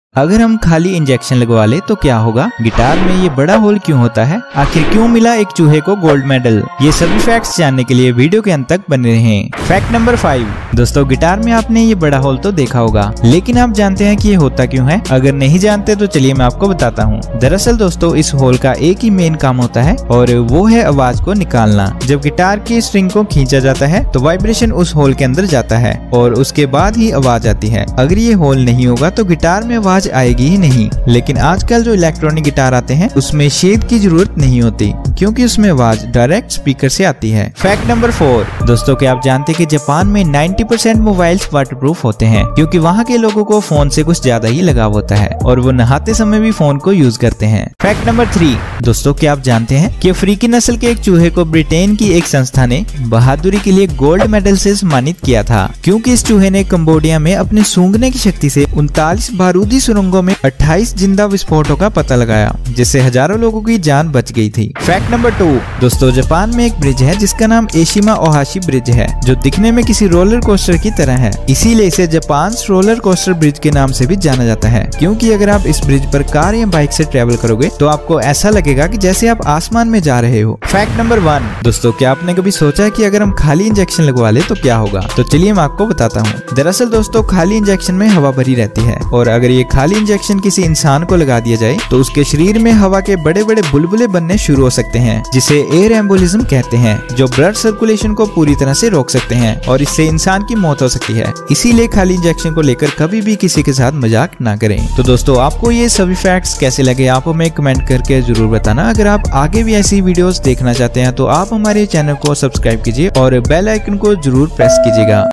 अगर हमें खाली Injection लगाया जाए तो क्या होगा || Facts || Factie Singh
दोस्तो आज मैंने इस वीडियो में 5 Random facts बताए हैं, जिसमे मैने ये भी बताया है की अगर हमें खाली Injection लगाया जाए तो क्या होगा । तो दोस्तो अगर आप भी ये सभी facts जानना चाहते हैं तो इस वीडियो को अंत तक देखिएगा।